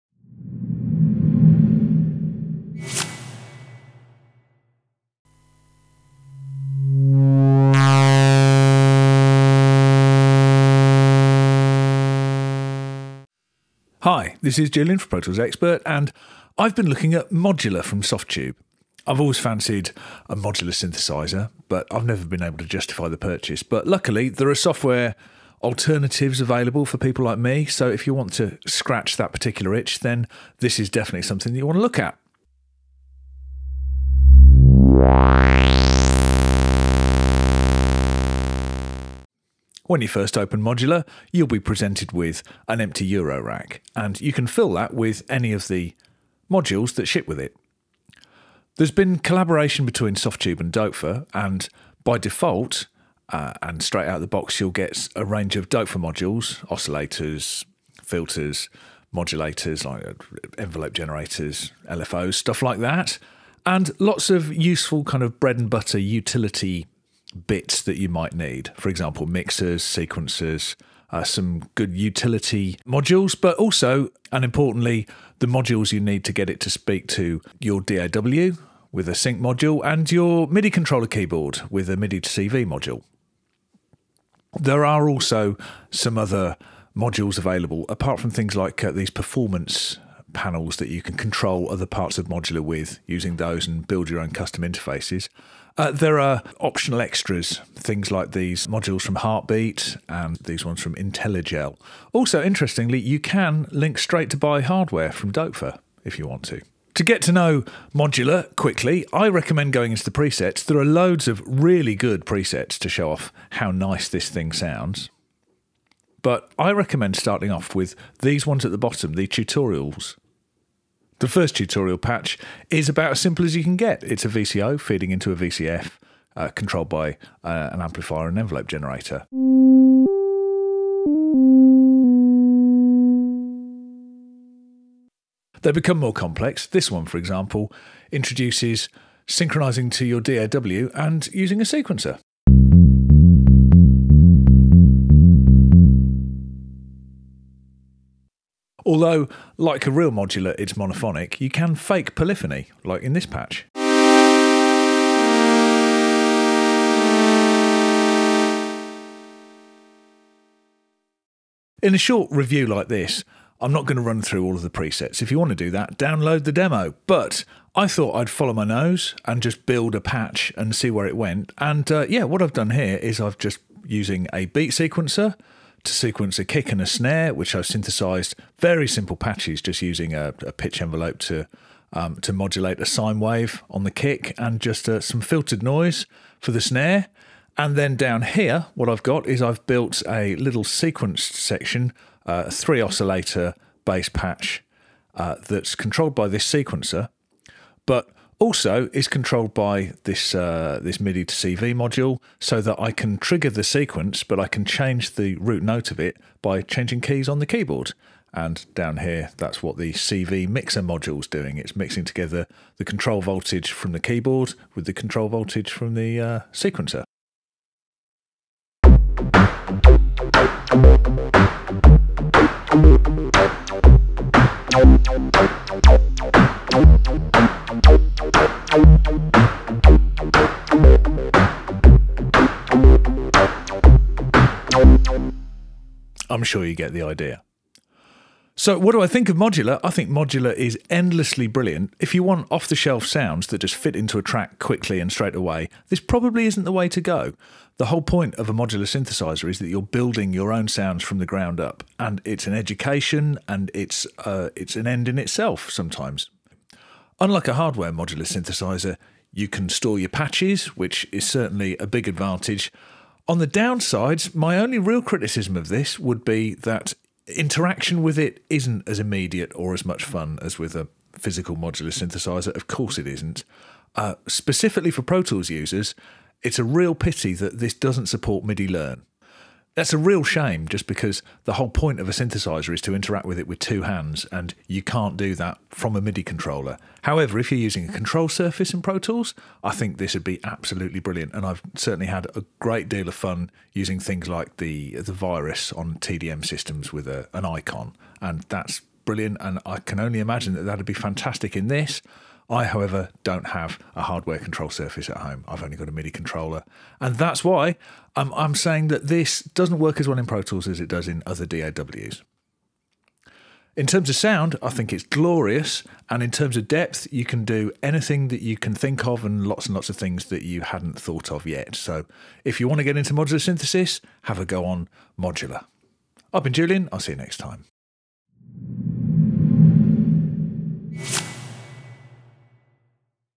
Review - Softube Modular
In this Pro Tools Expert review Julian Rodgers takes a looks at Modular by Softube